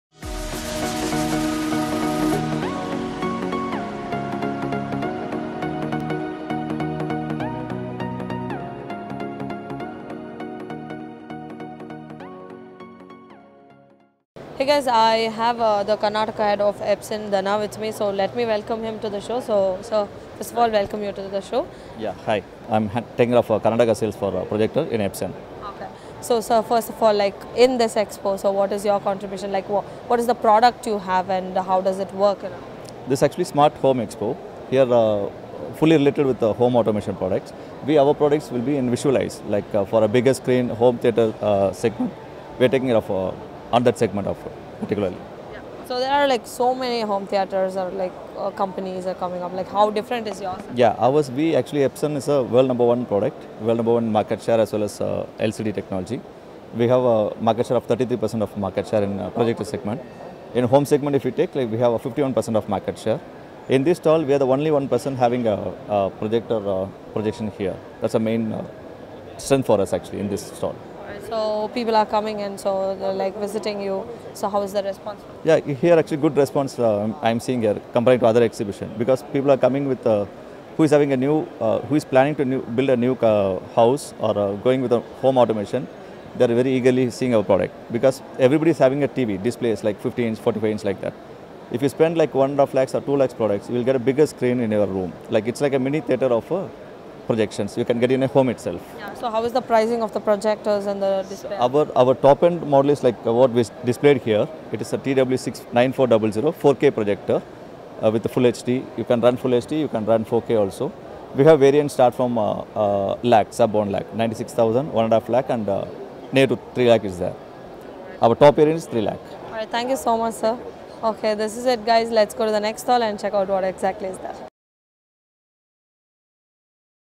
Dhananchezhian of EPSON to Prajwala of Smartsustain.in at Smart Home Expo 2019 at KTPO, Bengaluru
Redefining the world of technology EPSON has grown to become a leading digital imaging and printing definition in India. EPSON proved to be synonymous to innovations through their advanced technical and technological potential for lucid, meticulous processing.